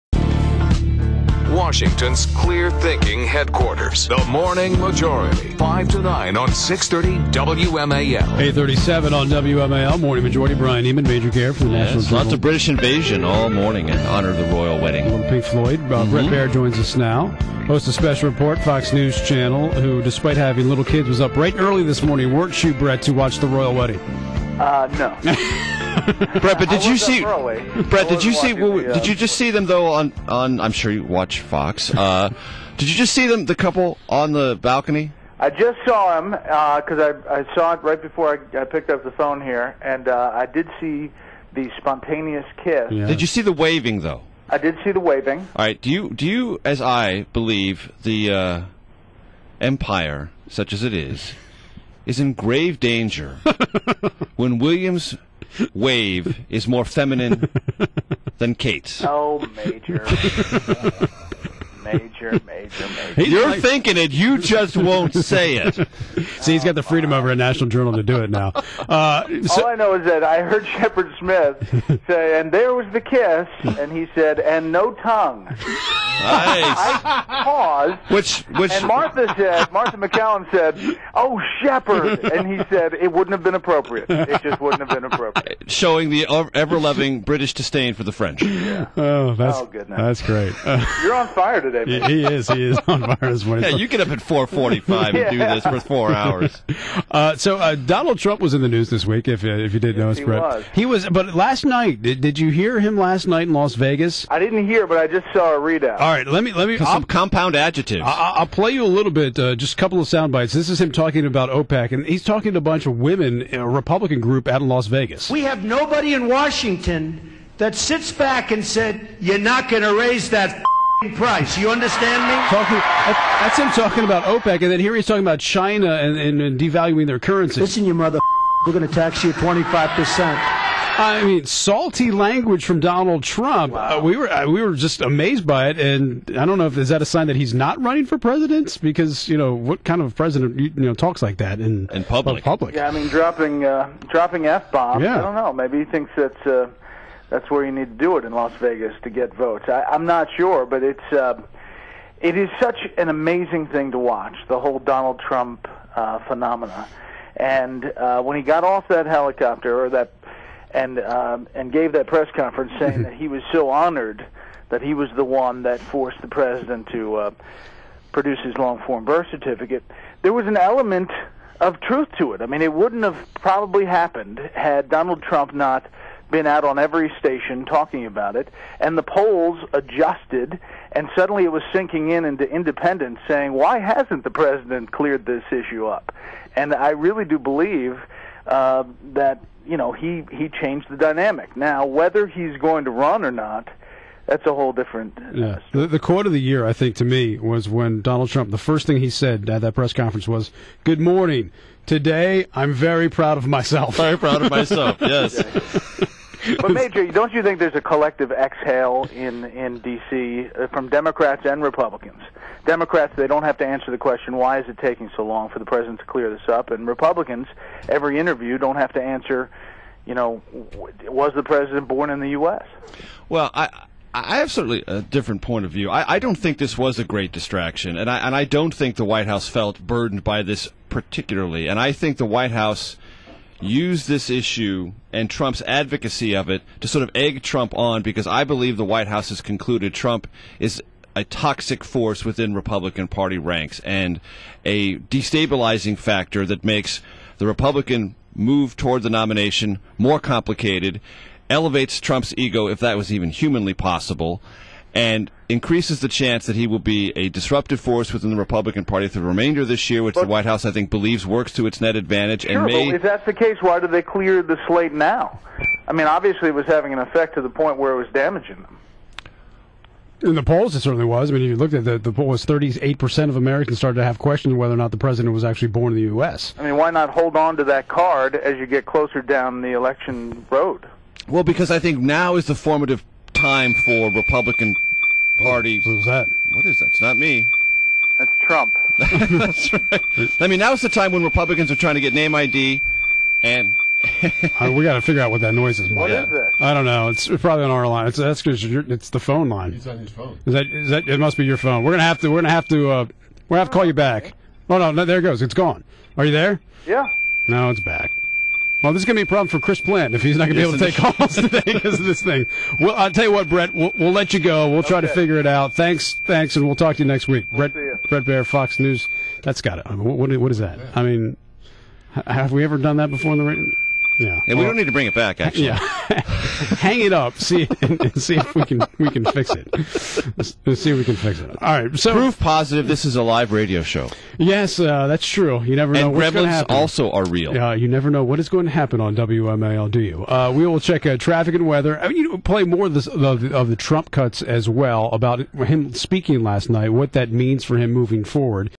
BRET BAIER on Royal Wedding
An elegant, tiara-bedecked Kate Middleton swept down the aisle to marry Prince William at Westminster Abbey as royal fans packed the streets of London, hoping to snatch a glimpse of a historic royal wedding expected to revitalize British monarchy.